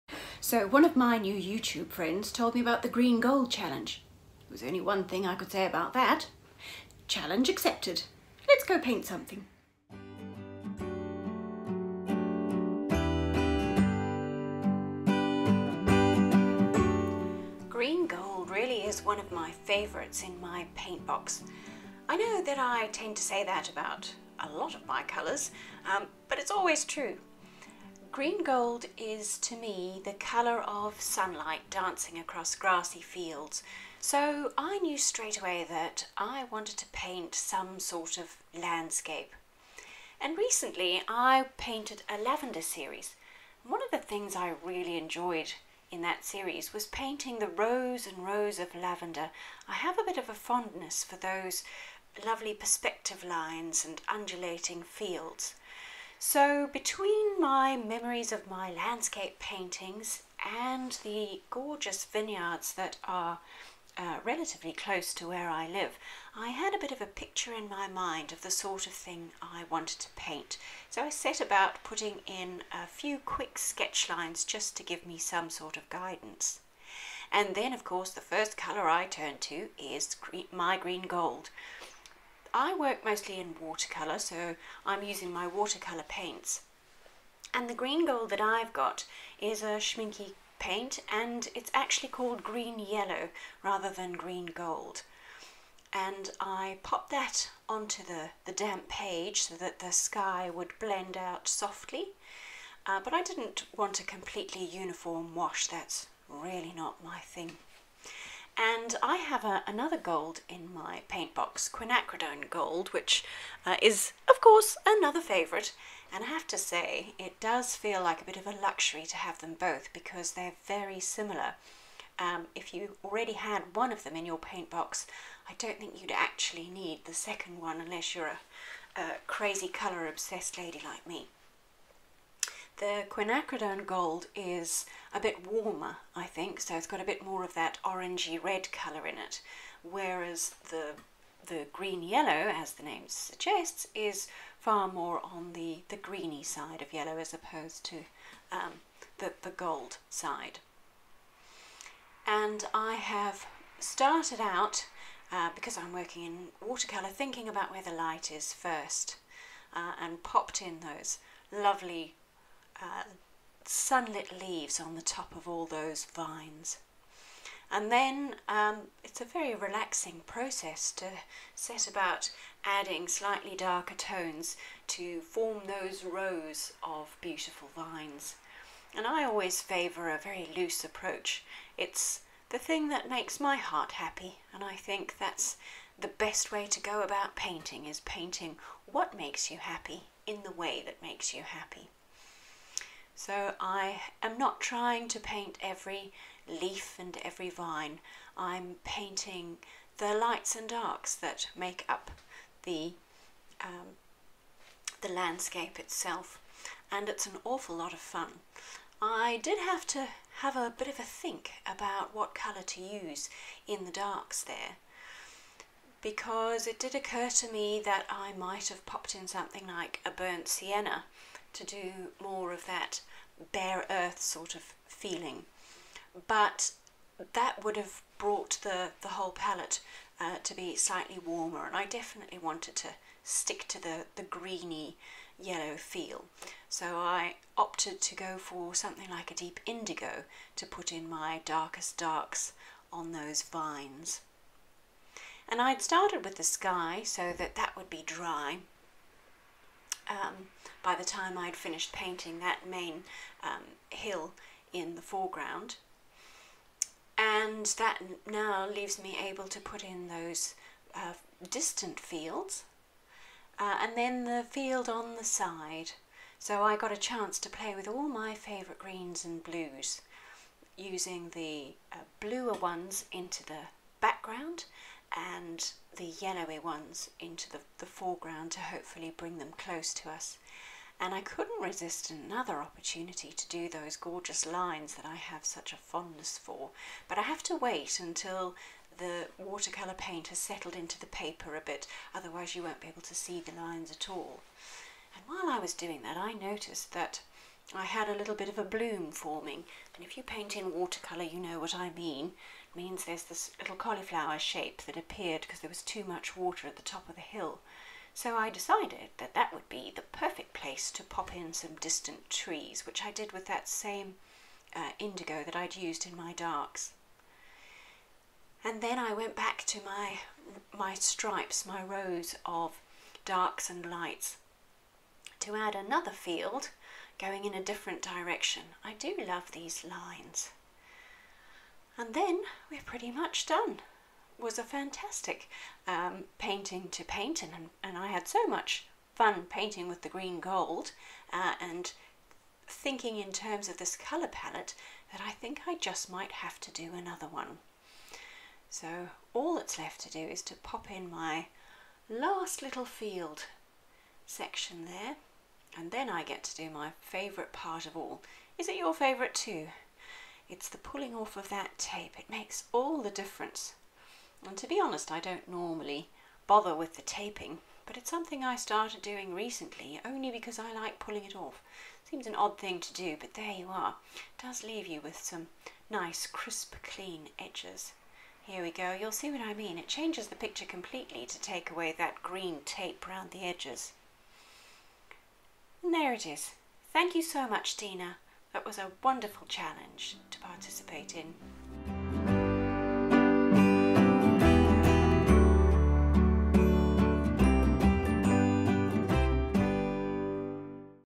Green gold challenge watercolour landscape kerrie woodhouse
GREEN GOLD CHALLENGE - WATERCOLOR LANDSCAPE TIME LAPSE PAINTING

This video is my contribution to  Dena Tollefson’s Green Gold Challenge. It’s the first challenge I have participated in on Youtube - thanks Dena! What a great idea.

Green Gold is one of my favourite colours (ok yes, I know I say that about most of them…) I think it is the colour of sunlight dancing on grassy fields. I chose a vineyard scene to make the most of this sunny, lively colour.

It was so much fun to do, I might just have to do another! Can’t wait to see everyone else’s Green Gold Challenge paintings!

RESOURCES AND LINKS MENTIONED IN THE VIDEO

I am painting with a Schmincke watercolour paint which is actually called  Green Yellow

READY TO START YOUR OWN PAINTING JOURNEY?

Looking to start your own watercolour journey? How about a free class?

SAY HELLO

Hope you enjoy watching my Green Gold Challenge watercolour painting!